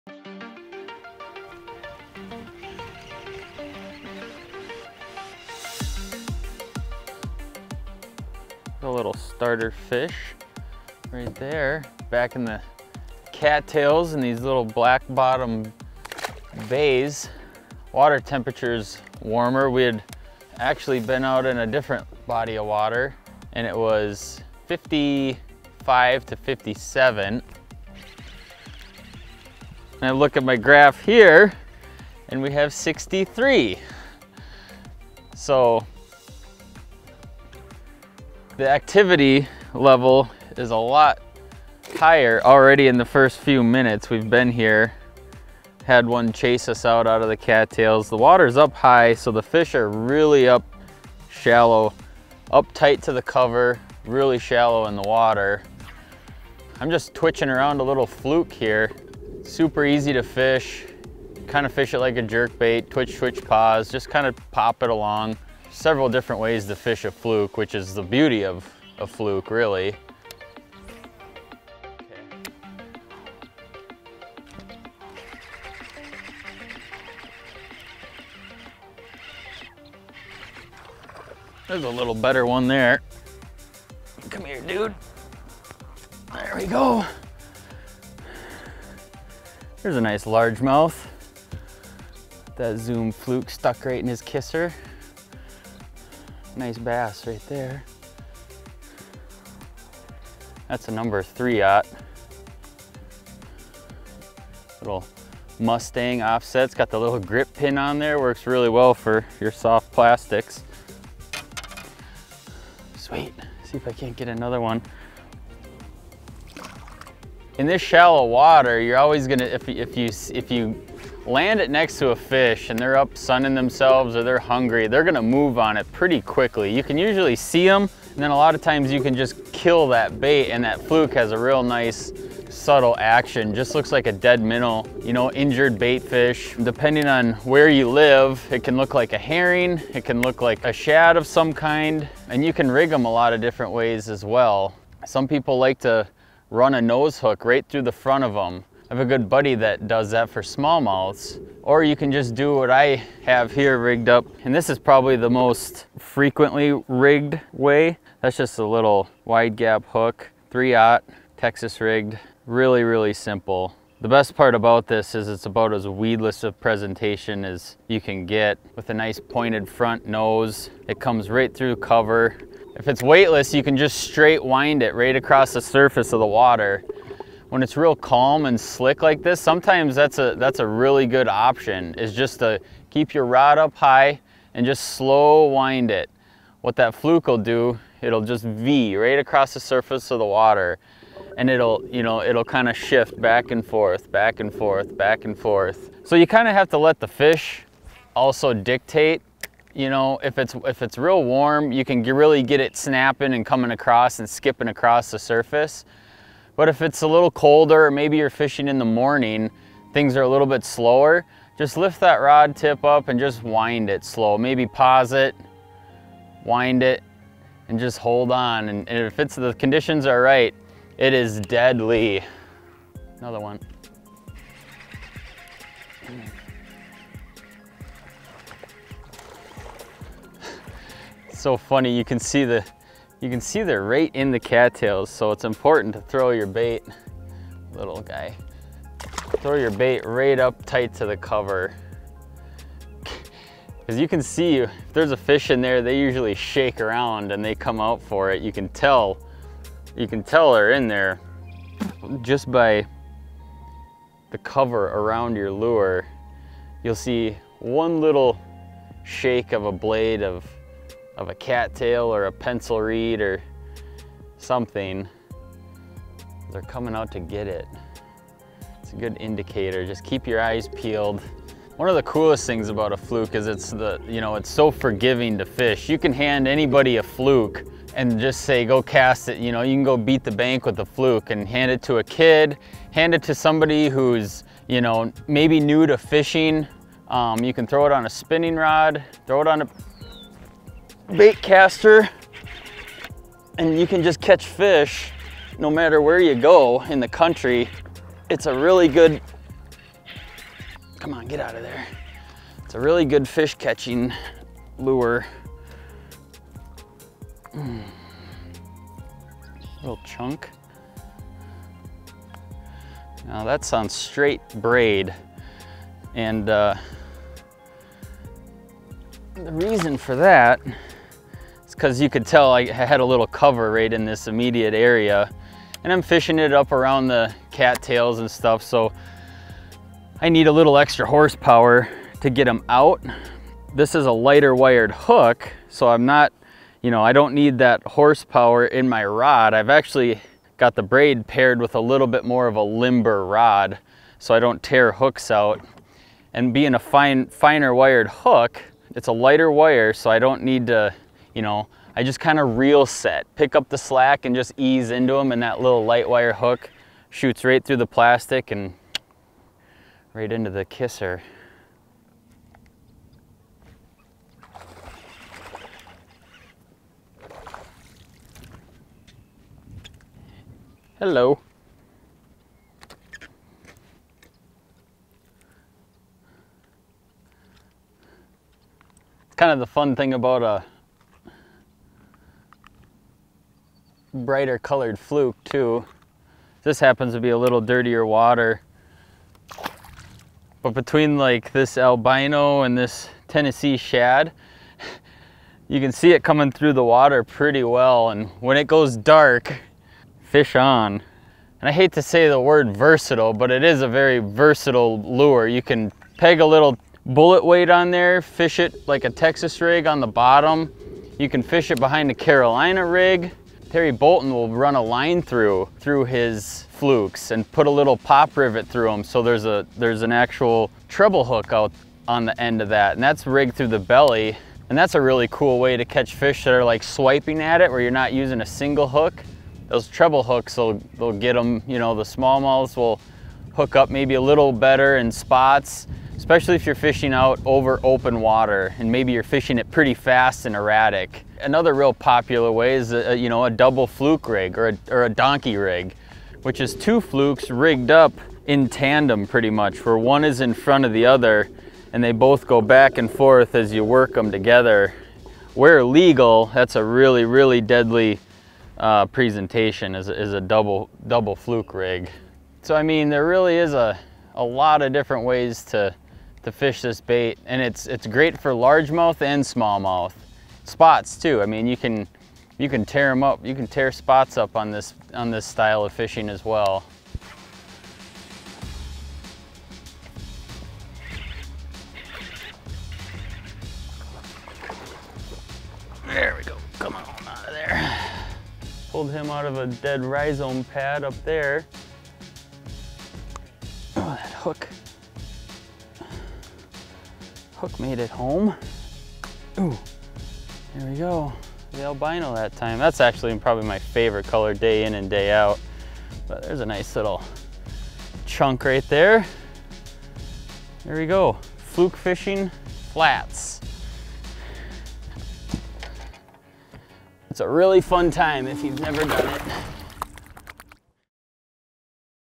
Tips for Fluke Fishing Bass on Grass Flats
Few bass baits provide the sheer versatility and fish-catching ability of a soft jerkbait. Fluke-style plastics like the ZOOM Super Fluke make the top of the list; Wired2Fish's Kyle Peterson demonstrates why in this action-packed video covering how to catch bass on the bank and scatted along extensive glass flats.

Warming and high water have sent droves of bass into the grassy shallows where Kyle is able to work a weedless Texas-rigged soft jerkbait with ease. Beyond fishing clean, the plastic imitates a myriad of baitfish, making it a stealthy imitator that kids to seasoned pros alike can catch largemouth, smallmouth and spotted bass on.

Peterson shares the details of his rod, reel and line setup, as well as his favorite color for the situation. He also hits on some other excellent Fluke rigging tips such as a proven treble hook line-through option as well as the double fluke rig (aka donkey rig)